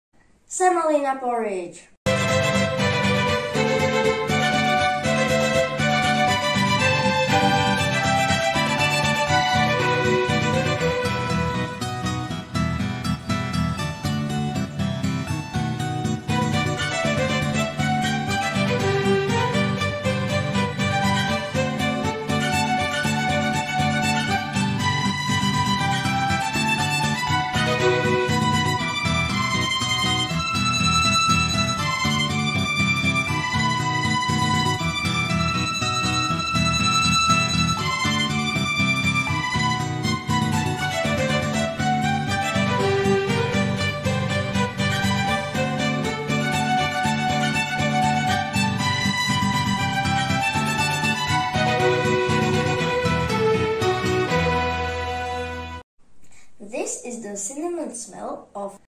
My favourite semolina porridge 🥣
Ingredients for 1 portion:

3 tablespoons semolina
300ml milk
20g butter
2 tablespoons cane sugar

Your favorite fruitThere is only one basic rule when making this porridge: for one tablespoon of semolina, you will need 100 ml of milk.

While stirring constantly, cook the semolina in the milk.
Pour the porridge onto a plate.
Place the butter on the warm porridge  and let it melt.
Mix the sugar with cinnamon and sprinkle this mixture over the porridge.
Serve with your favorite fruit.